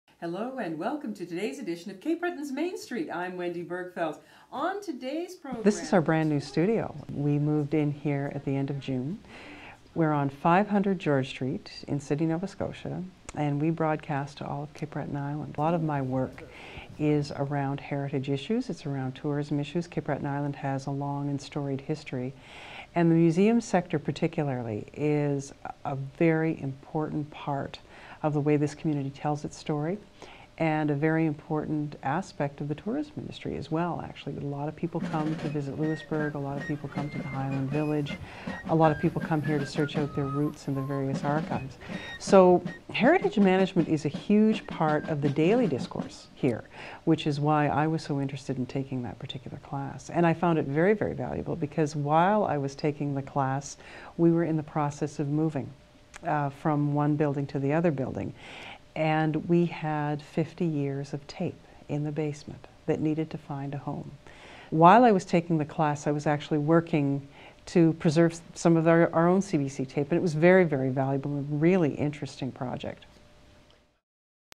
Wendy Bergfeldt: Linking the past to the present with AU heritage management studies
Here's how Wendy Bergfeldt connected a heritage management course from Athabasca University to her work as a producer for CBC Nova Scotia and her life on Cape Breton Island. The heritage management course is part of a Master of Arts degree in cultural and community studies that Bergfeldt is completing at AU.